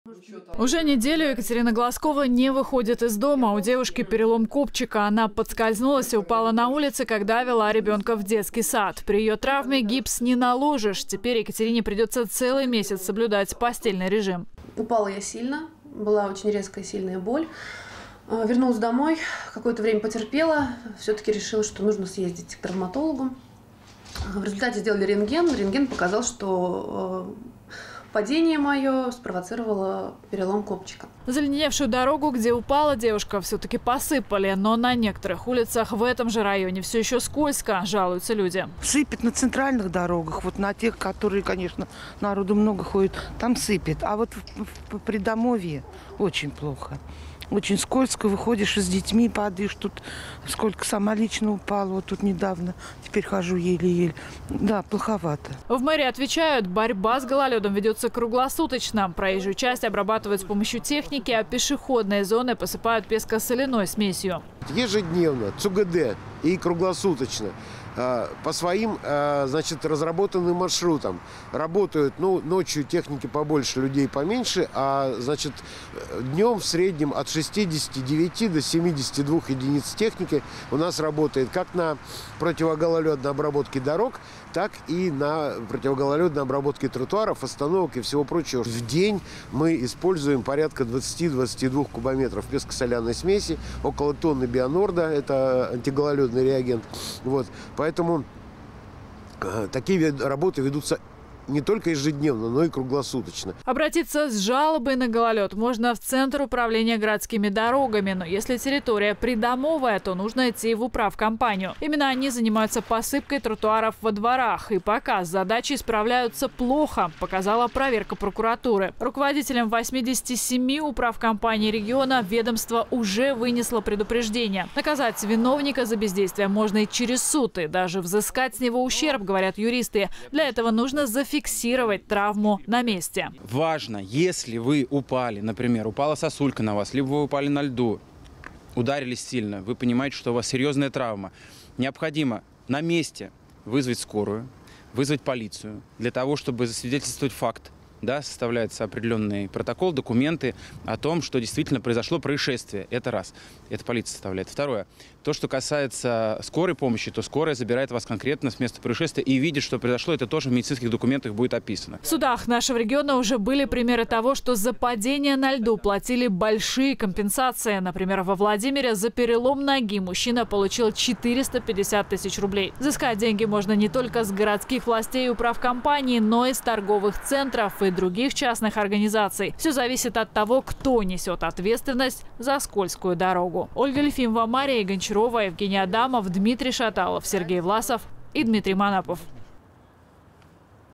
Первые жертвы владимирского льда
Жительница Владимира получила травму на скользком тротуаре. Она провожала ребенка в детский сад и упала прямо на лед. Другие горожане тоже жалуются на пешеходные дороги. Песком посыпано не везде, а там, где он есть – все равно скользко. Уже неделю Екатерина Глазкова не выходит из дома. У девушки перелом копчика. Она поскользнулась и упала на улице, когда вела ребенка в детский сад. При ее травме гипс не наложишь. Теперь Екатерине придется целый месяц соблюдать постельный режим.

    — Я сразу почувствовала, что упала я сильно, была очень резкая сильная боль, вернулась домой, какое-то время потерпела, все-таки решила, что нужно съездить к травматологу. В результате сделали рентген, рентген показал, что падение мое спровоцировало перелом копчика, - вспоминает Екатерина Глазкова, жительница г. Владимира.

Заледеневшую дорогу, где упала девушка, все-таки посыпали. Но на некоторых улицах в этом же районе – все еще скользко – жалуются люди.

    — Сыпят на центральных дорогах, вот на тех, которые, конечно, народу много ходит, там сыпят, а вот в придомовые – очень плохо. Очень скользко, выходишь с детьми, падаешь тут, сколько сама лично упала вот тут недавно, теперь хожу еле-еле, да, плоховато, - говорит Татьяна Андропова, жительница г. Владимира

В мэрии отвечают – борьба с гололедом ведется круглосуточно. Проезжую часть обрабатывают с помощью техники, а пешеходные зоны посыпают песко-соляной смесью.

    — Ежедневно ЦУГД и круглосуточно, по своим, значит, разработанным маршрутам, работают, ночью техники побольше, людей поменьше, а значит, днем, в среднем, от 69 до 72 единиц техники у нас работает. В день мы используем порядка 20-22 кубометров песко-соляной смеси, около тонны бионорда – это антигололедный реагент, вот, поэтому такие работы ведутся не только ежедневно, но и круглосуточно, - сообщил Игорь Ефремов, заместитель начальника управления по связям с общественностью и СМИ администрации г. Владимира.

Обратиться с жалобой на гололед можно в центр управления городскими дорогами. Но если территория придомовая - то нужно идти в управкомпанию. Именно они занимаются посыпкой тротуаров во дворах. И пока с задачей справляются плохо, показала проверка прокуратуры. Руководителям 87 управкомпаний региона ведомство уже вынесло предупреждения. Наказать виновника за бездействие можно и через суд. И даже взыскать с него ущерб, говорят юристы. Для этого нужно зафиксировать травму на месте.

    — Важно, если вы упали, например, упала сосулька на вас, либо вы упали на льду, ударились сильно, вы понимаете, что это серьезная травма, необходимо на месте вызвать скорую, вызвать полицию для того, чтобы засвидетельствовать факт, да, составляется определенный протокол, документы о том, что действительно произошло происшествие. Это раз. Это полиция составляет. Второе, то, что касается скорой помощи, то скорая забирает вас конкретно с места происшествия и видит, что произошло – это тоже в медицинских документах будет описано, - объясняет Роман Ардыкуца, юрист.

В судах нашего региона уже были примеры того, что за падение на льду платили большие компенсации. Например, во Владимире за перелом ноги мужчина получил 450 тысяч рублей. Взыскать деньги можно не только с городских властей и управкомпаний, но и с торговых центров и других частных организаций. Все зависит от того, кто несет ответственность за скользкую дорогу.